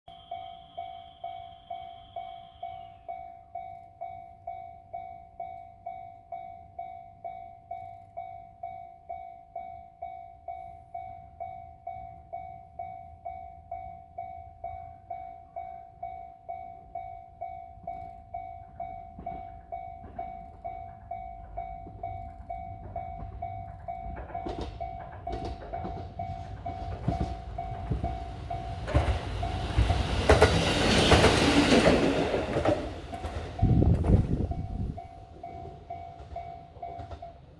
2024/1/6 普通列車(キハ220)走行@日田～光岡間
2024/1/6　JR久大本線日田～光岡間を走行する久留米行き普通列車(キハ220_1両)を撮影しました。